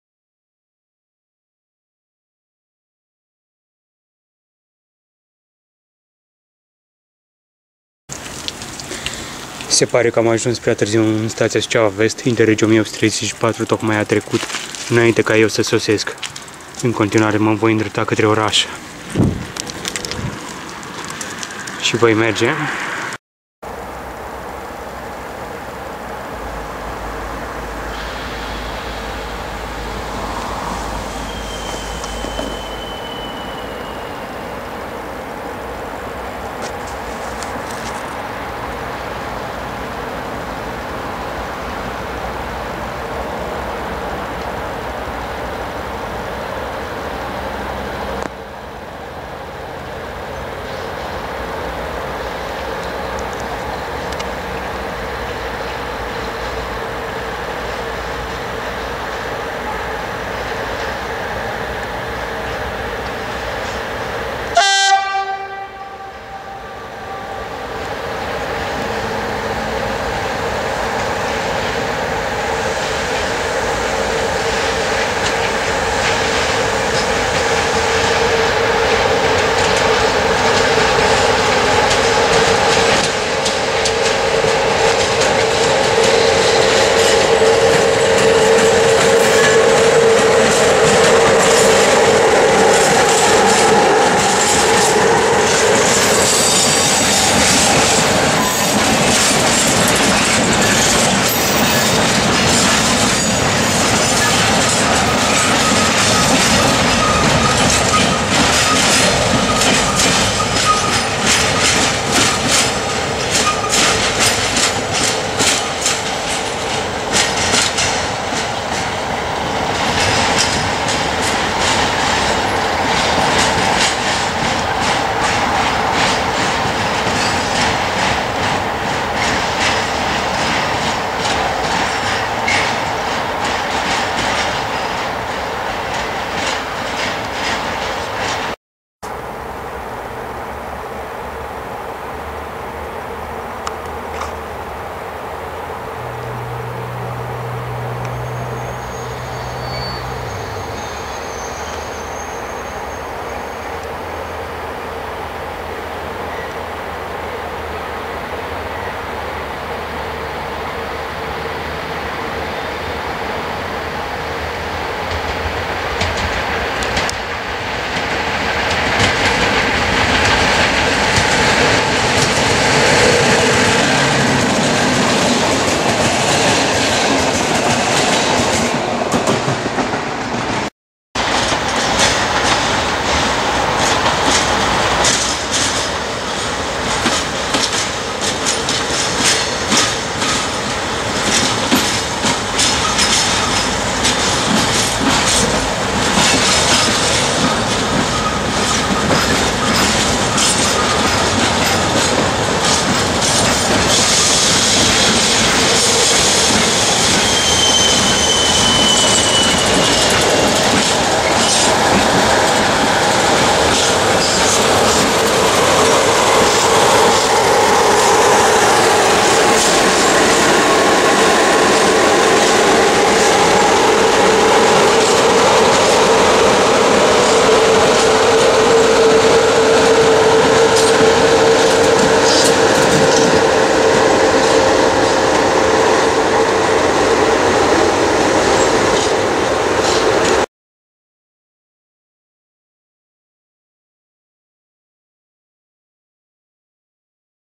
Activitate feroviara in statia Suceava - 04.11.2012
1. Plecarea din Suceava Vest dupa ce am prins nici un tren.
2. Manevra GFR cu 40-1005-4
3. R5506 Suceava Nord - Pascani cu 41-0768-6 a dep. Iasi
4. Manevra GFR 2